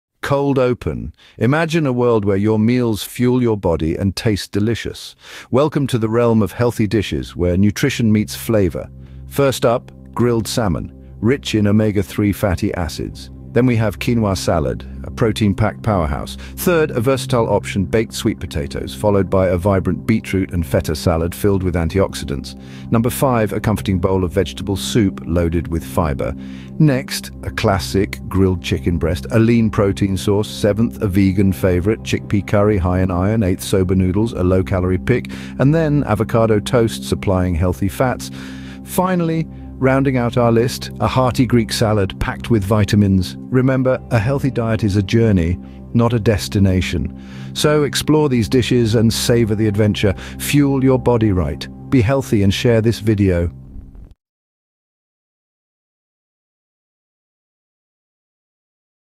top 10 healthy dishes for a healthy diet
top 10 healthy dishes for a healthy diet. Cold Discovery. Imagine a world where your meals nourish your body and taste delicious. Welcome to the realm of healthy dishes where nutrition meets flavor. First, grilled salmon, rich in omega-three fatty acids. Next, a quinoa salad, rich in protein. Third, a versatile option is a baked sweet potato. This is followed by a bright beet and feta salad filled with antioxidants. Fifth is a comforting bowl of fiber-rich vegetable soup. Next is a classic, grilled chicken breast, a source of lean protein. Seventh is a vegan favorite, chickpea curry, high in iron. Eighth, soba noodles, a low-calorie dish. And finally, avocado toast is a source of healthy fats. And finally, completes our list with a hearty Greek salad full of vitamins. Remember that healthy eating is a journey, not a destination. So try these dishes and enjoy the adventure. Fuel your body the right way.